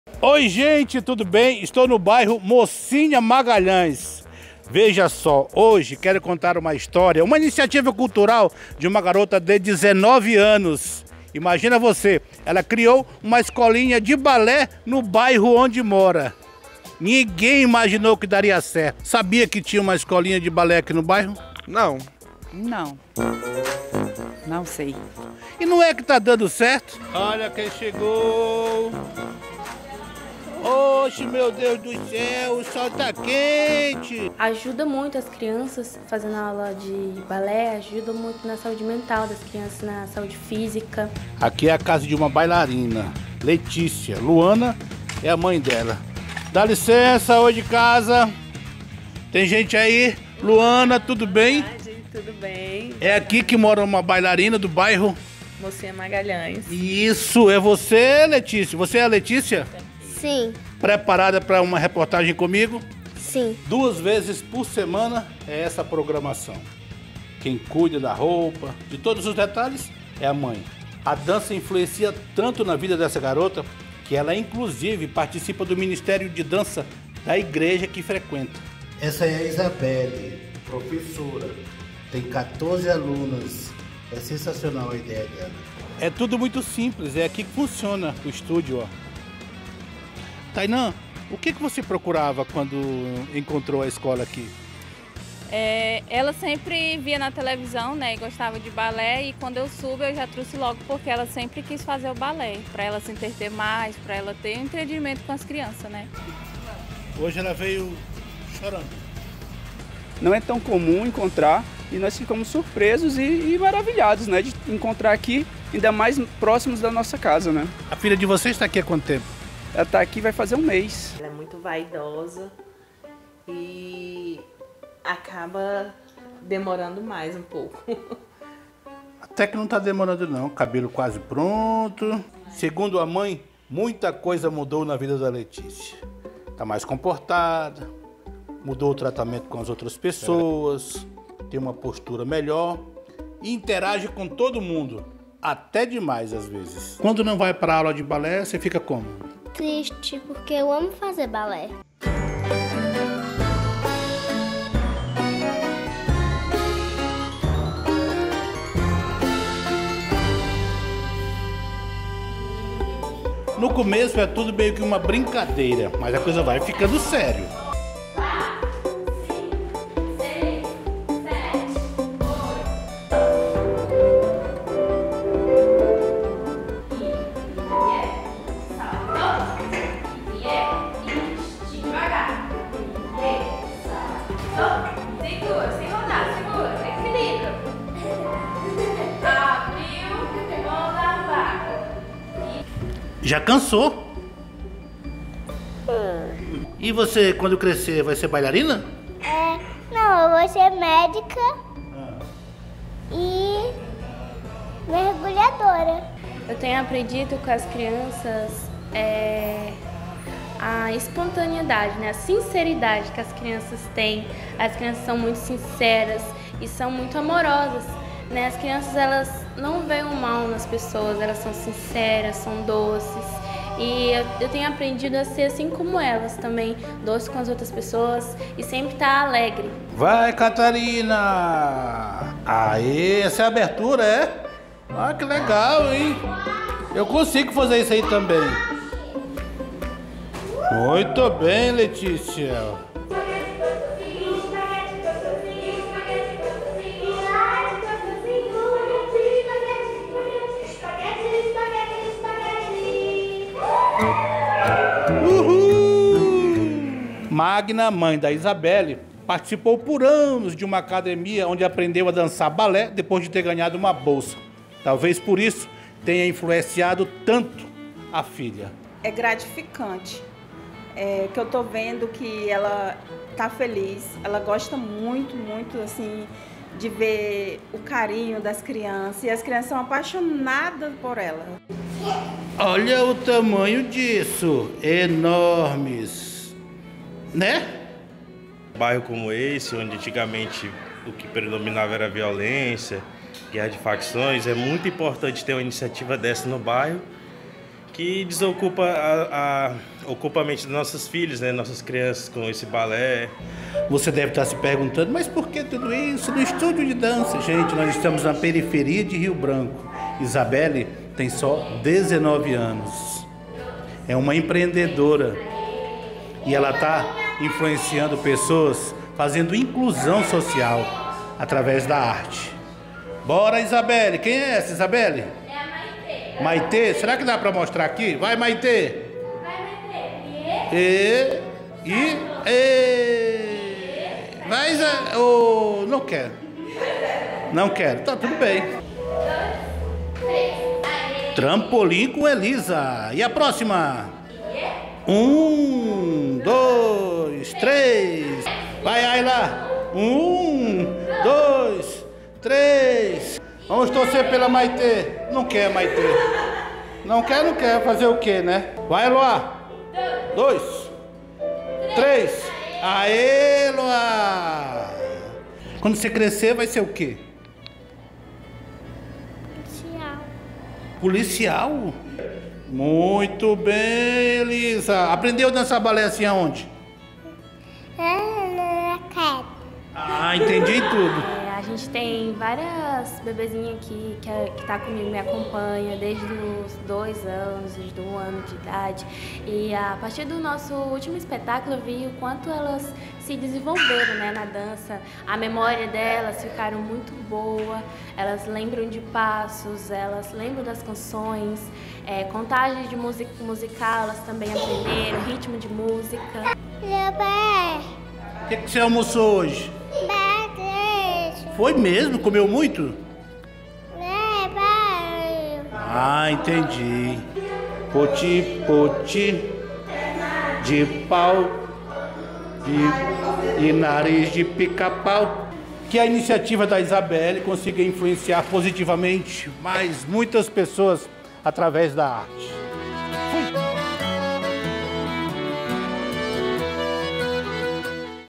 Escolinha de balé mobiliza crianças e promove inclusão no Mocinha Magalhães
Prática considerada incomum entre a maior parte dos jovens acreanos, o balé está presente no estado e não apenas nos lugares mais “nobres”. Uma escolinha localizada no bairro Mocinha Magalhães faz sucesso entre crianças de várias idades e promove inclusão social na periferia de Rio Branco.

A ideia de criar a escolinha foi de Isabelly Fernandes, de 19 anos de idade, inspirada pela mãe, Magna, que quando jovem também foi praticante de balé por meio de uma bolsa de estudos.

Ninguém imaginou que daria certo e, inclusive, alguns moradores não sabem que funciona a escolinha no bairro. Mas ela está lá e, melhor ainda, está dando muito certo, como mostra o videomaker do ac24horas, Kennedy Santos.

Na casa simples e de poucos cômodos, um é muito especial e foi transformado em estúdio de dança. Duas vezes por semana, duas turmas se revezam na pequena sala. O ar-condicionado ajuda a melhorar a temperatura ambiente, que além de uma pequena mesa, guarda no cantinho um bebedouro com água sempre boa.

Os pais, cuidadosos com o desenvolvimento dos filhos, chegam de bicicleta, carros, a pé e até em motos. A mensalidade quase irrisória de 70 reais por mês, se for levado em conta o poder da arte, nem de longe é motivo de reclamação.